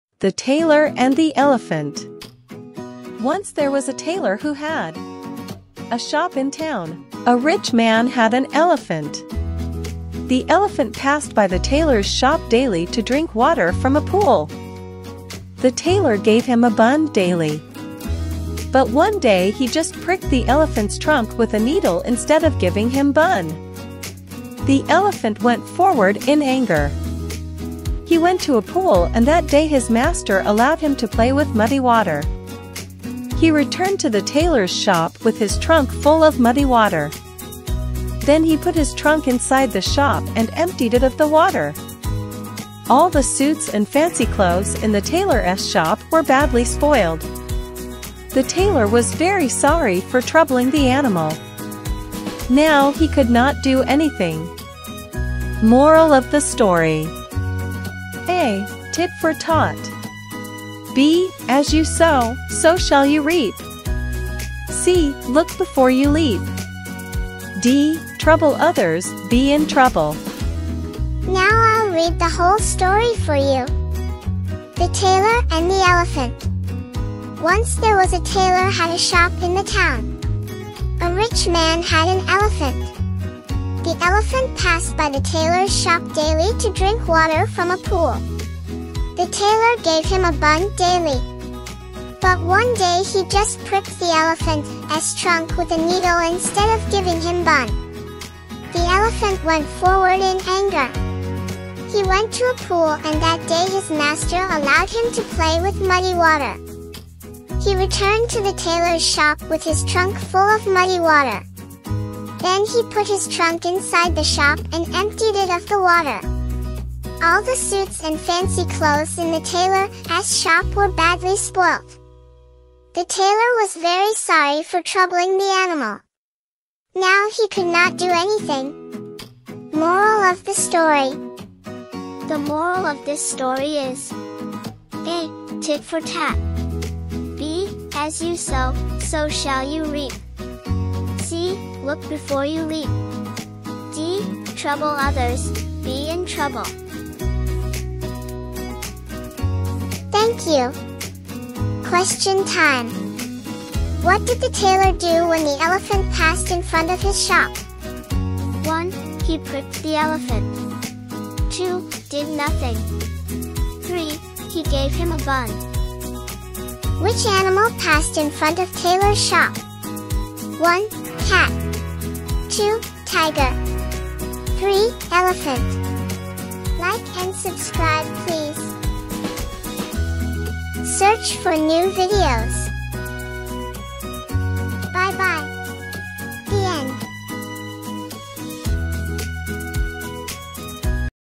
The Tailor and the Elephant
You do bad to other it come back to you, what goes around comes around